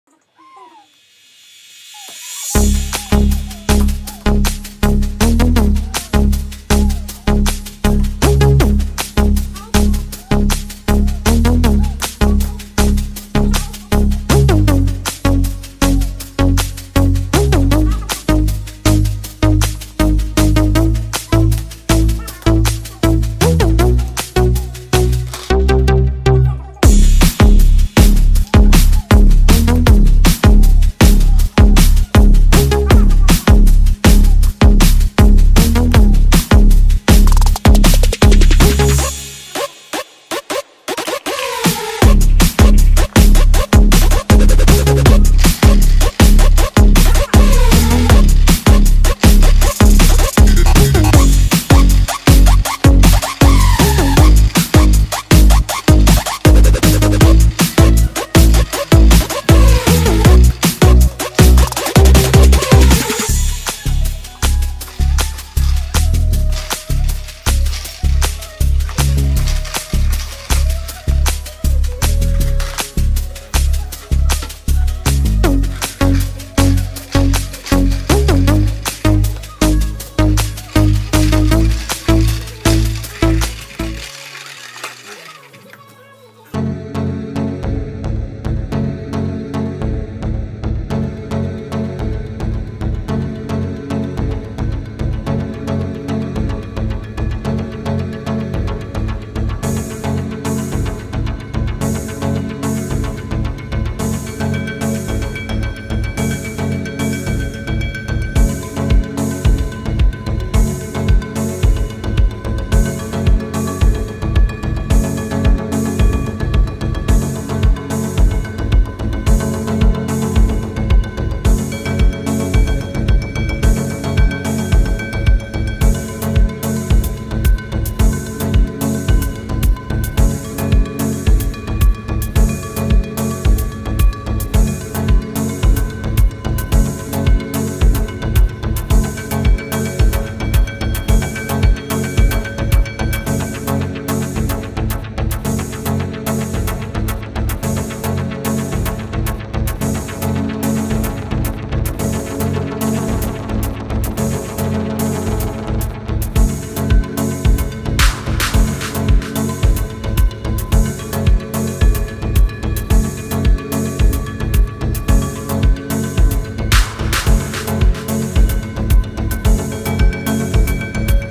আমার মেয়ে এভাবেই সারা ঘরে ঘোরাঘুরি করছে।blog #16
My daughter is moving around the house like this.